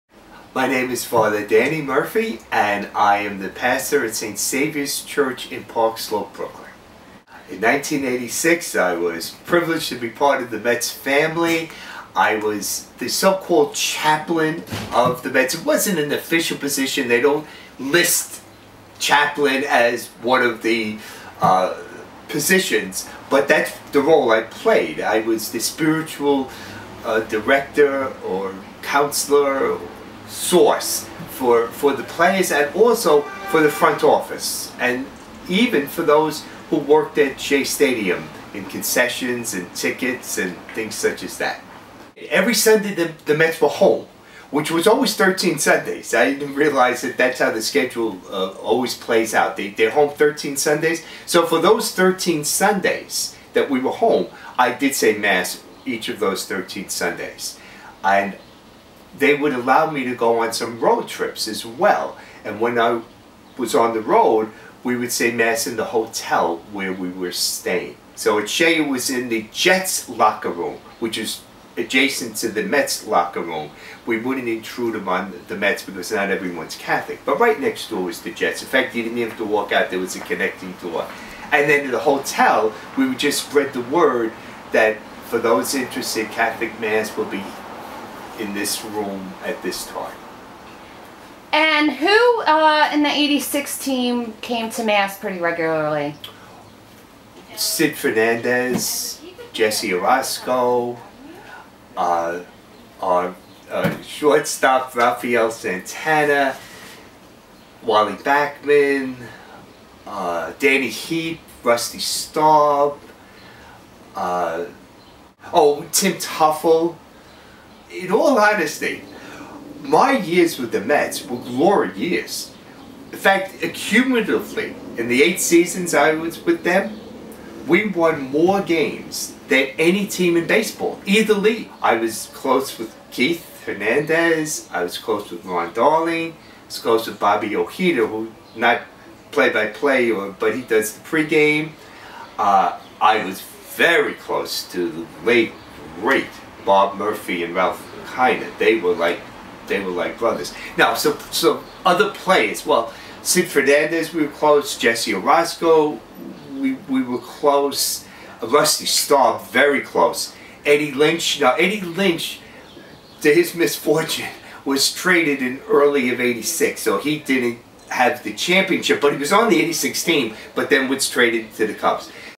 '86 Mets Chaplain, Father Daniel Murphy
Father Daniel Murphy (no relation) talks about some surprisingly devout players.